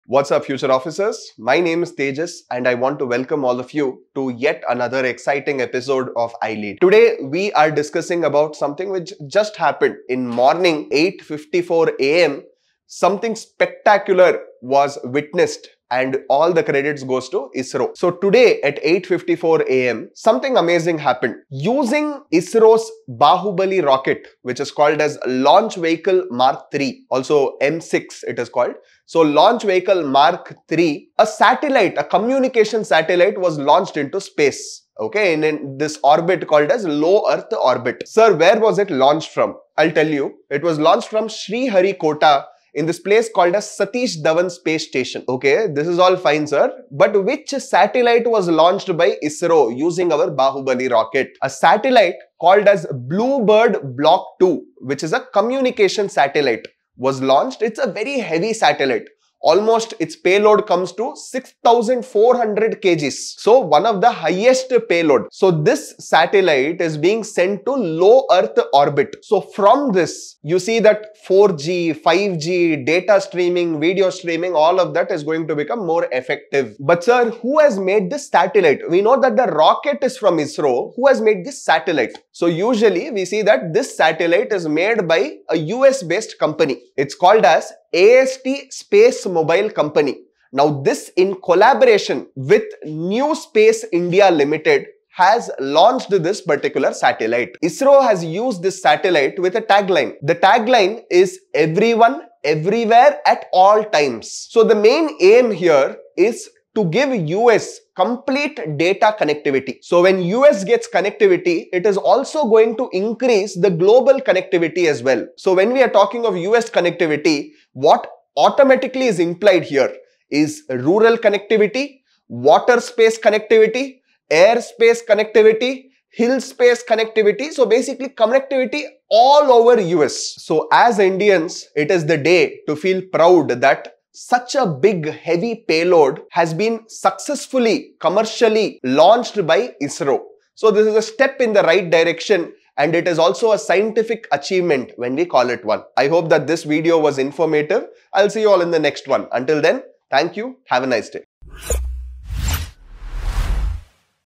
ISRO Launches Bluebird Block 2: A Giant Leap for Global Connectivity | iLead by Tejas sir
In this exciting episode of iLead, Tejas Sir takes us through ISRO’s latest scientific achievement – the successful launch of the Bluebird Block 2 communication satellite. The satellite, launched at 8:54 AM today using the powerful Launch Vehicle Mark 3 (Bahubali), weighs a whopping 6400 kgs, making it one of the heaviest payloads launched by ISRO.

Watch as Tejas Sir explains:

The significance of ISRO's Bahubali rocket and its launch from Sriharikota’s Satish Dhawan Space Station.

The Bluebird Block 2 satellite’s impact on global connectivity, including 4G, 5G, and data/video streaming improvements worldwide.

How US-based AST Space Mobile, in collaboration with New Space India Limited, has designed this satellite, aiming to provide complete data connectivity across rural, water, air, and hill spaces in the US.

This is a huge milestone for India, with ISRO making remarkable strides in global satellite communication, and Bluebird Block 2 is a game-changer for rural and global connectivity.